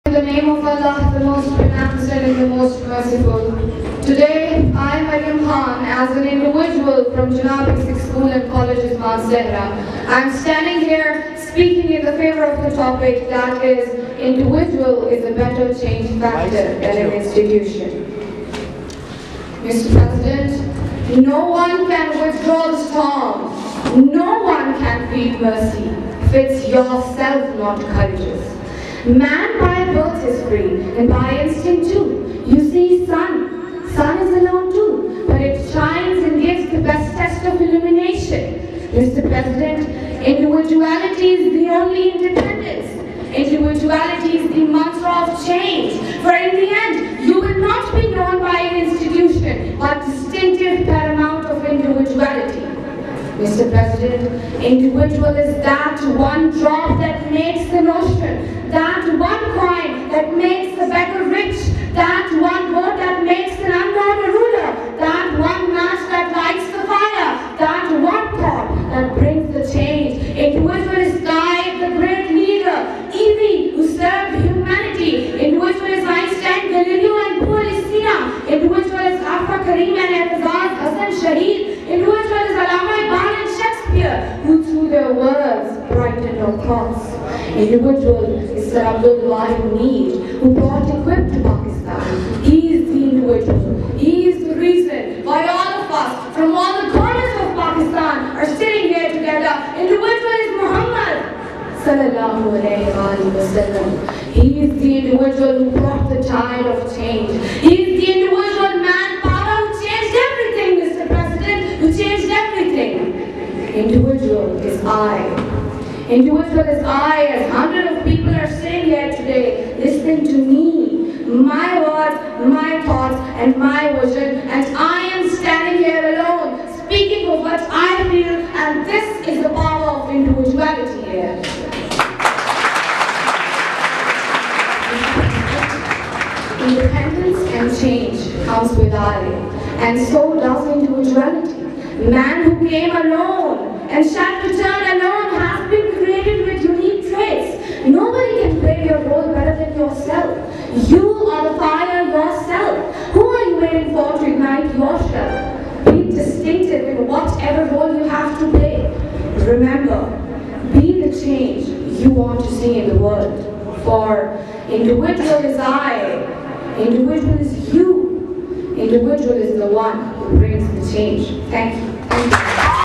DEBATE: in the favour of topic "Individuality is a better change factor than an institution"
Debate of our student “Maryum Faurez” in the favour of the topic "Individuality is a better change factor than an institution". Same debate was delivered in the 10th National Convention on Students Quality Circle on which Jinnah Basic School & Colleges Mansehra got 2nd position.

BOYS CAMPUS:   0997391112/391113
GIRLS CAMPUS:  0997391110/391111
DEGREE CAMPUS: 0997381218

| Jinnah Basic School & Colleges Mansehra |